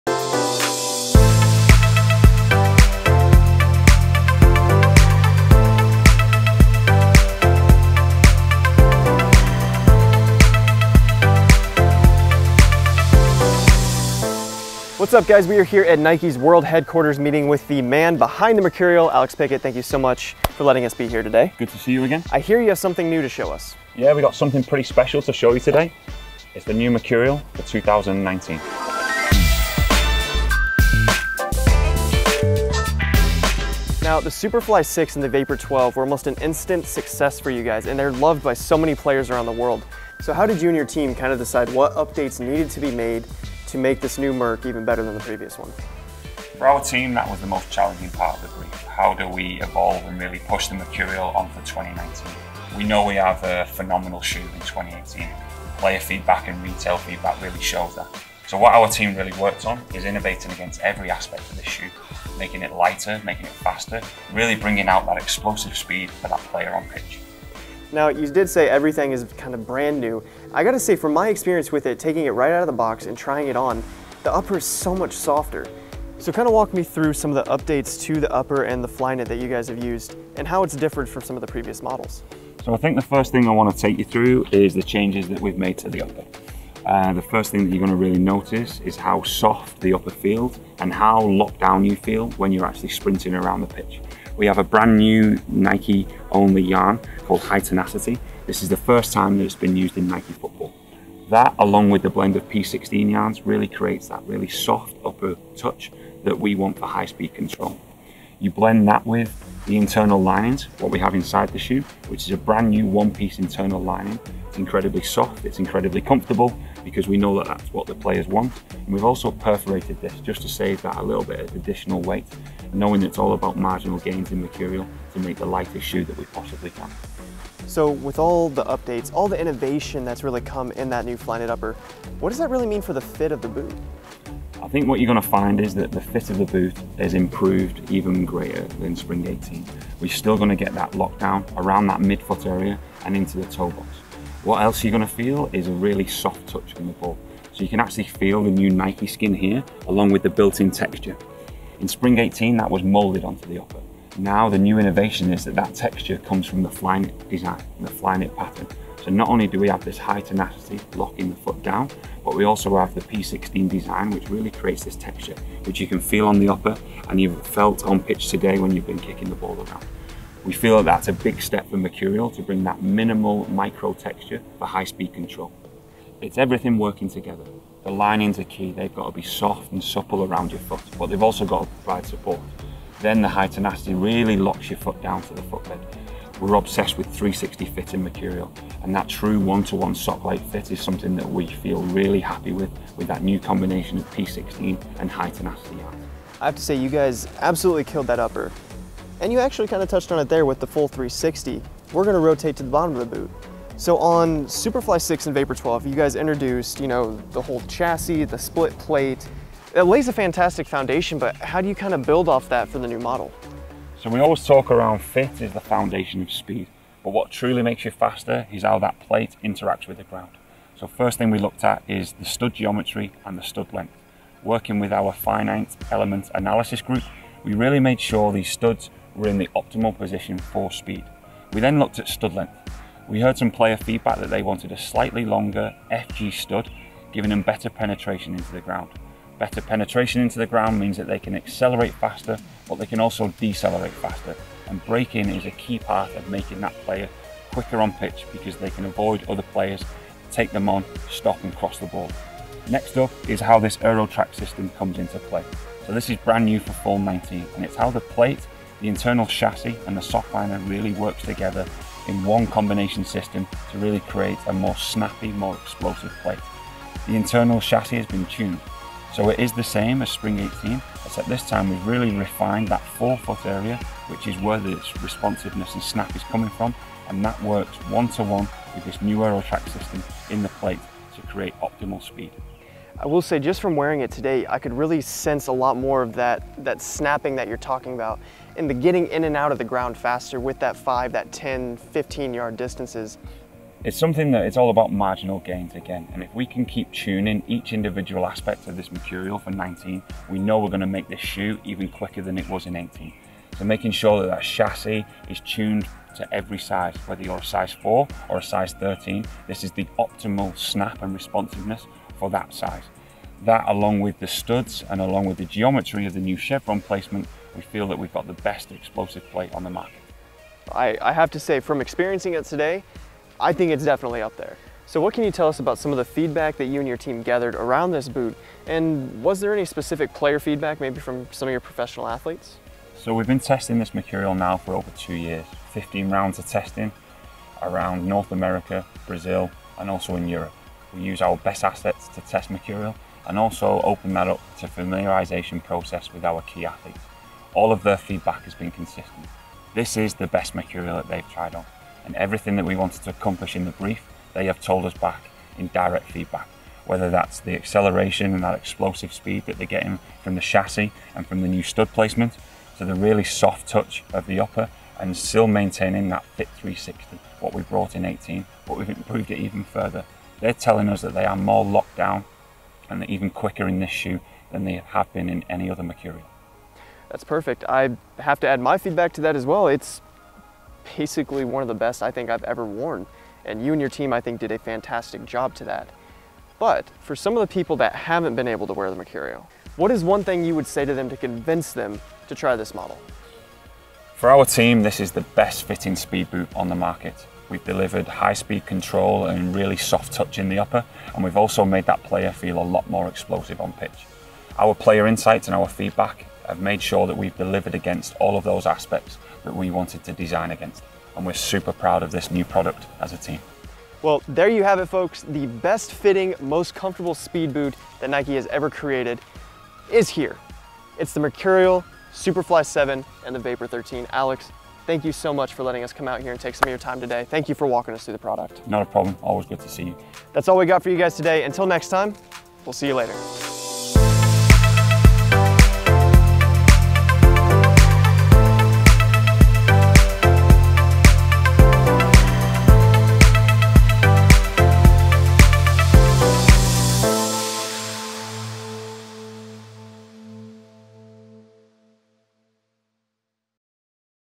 Nike Mercurial Superfly 7 and Vapor 13 Tech Dive | Nike Global Football Mercurial PLM Interview
The Mercurial Superfly 7 and Vapor 13 may look similar to their predecessors, but Nike made some serious strides forward in comfort, fit and traction. Learn about the latest Nike speed innovation from the man behind Mercurial, Alex Pickett.